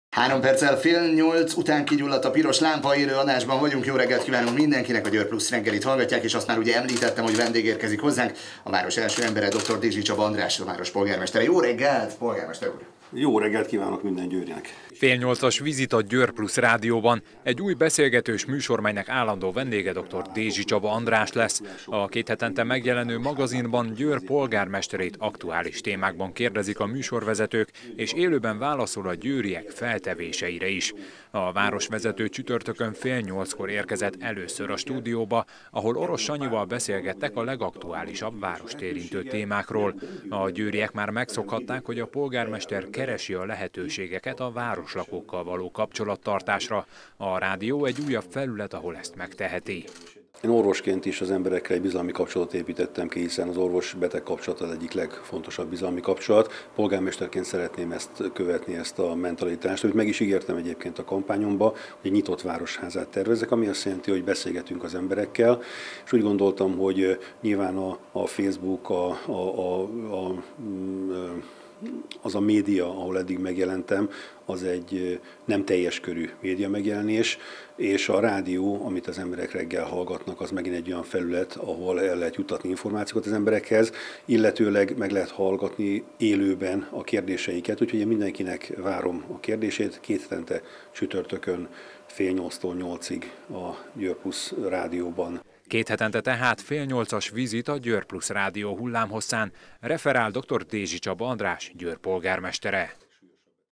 Kéthetente a rádióban vizitál dr. Dézsi
Fél nyolcas vizit címmel jelentkezik kéthetente a Győr+Rádió új beszélgetős műsora. Bár a vendég egy doktor, még sem egy egészségügyi magazinról van szó. Győr polgármestere látogat, ezentúl rendszeresen a rádió stúdióba, ahol a műsorvezetők kérdései mellett, a betelefonáló győriek feltevéseire is válaszol, élőben. Dr. Dézsi Csaba András az első vizit alkalmával elmondta, a rádió egy újabb olyan felület, ahol kapcsolatba lehet lépni az itt élőkkel.